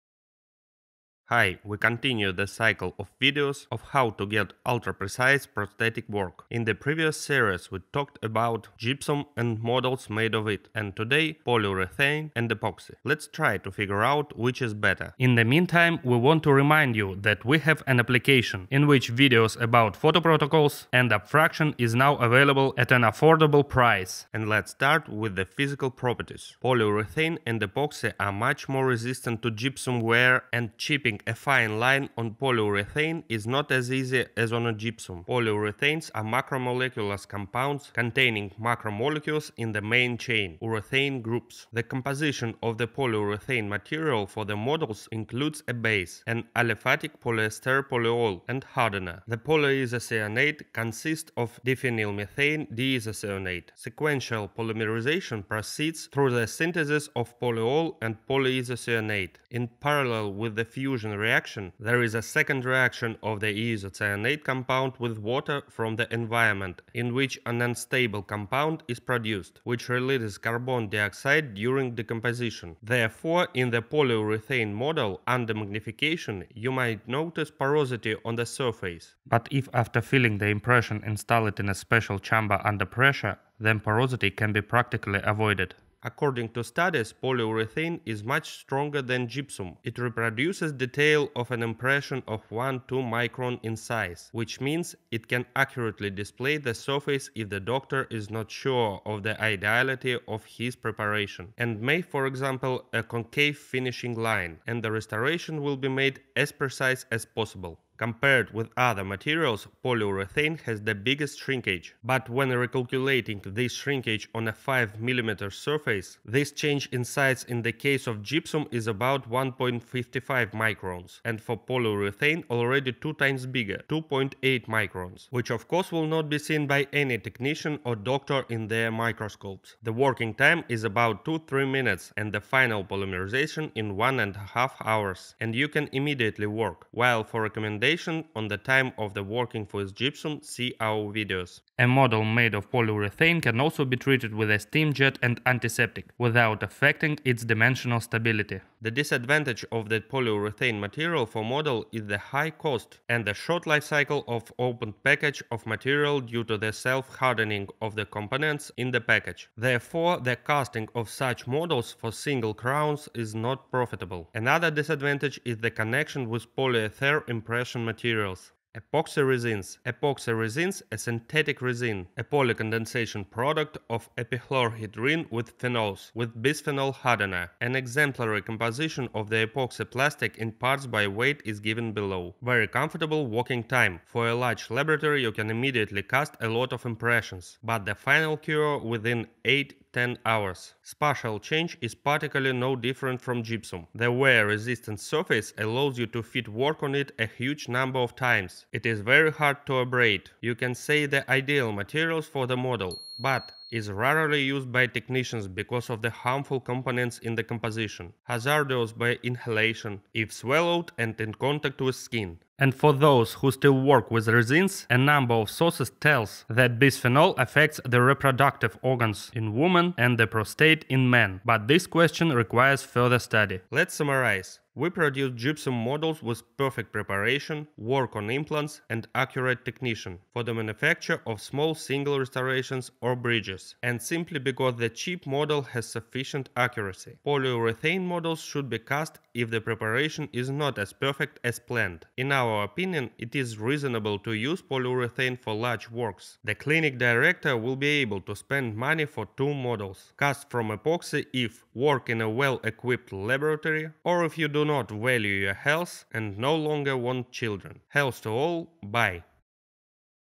How to choose material for the model? / How to Dentist
Epoxy resin or Polyurethane? They don’t talk about such materials at the university, and many doctors don’t know about them. In this video we will discuss with you alternative materials for casting working models, you will find out which one is more accurate, and some nuances for choosing material for the working model.